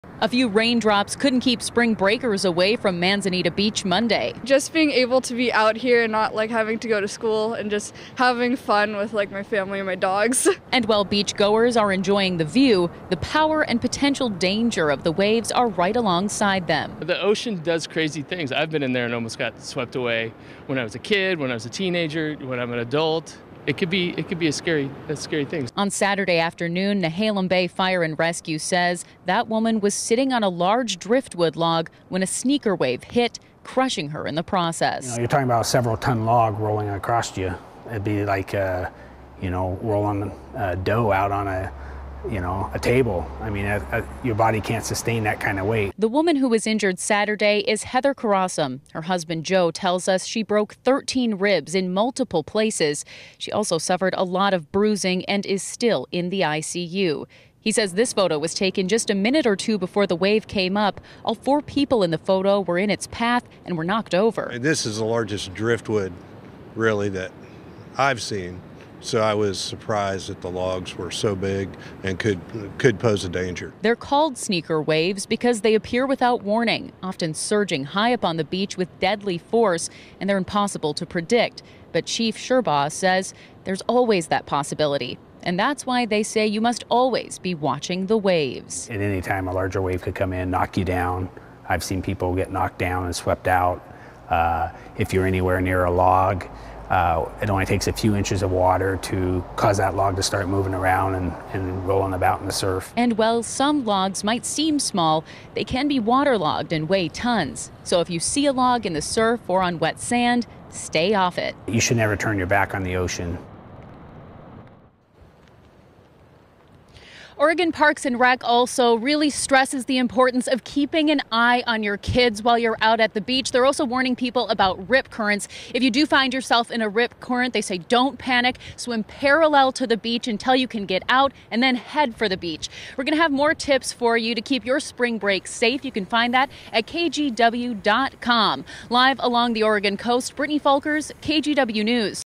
‘Never turn your back on the ocean’
A woman was seriously injured after a sneaker wave knocked a log into her on the Oregon Coast.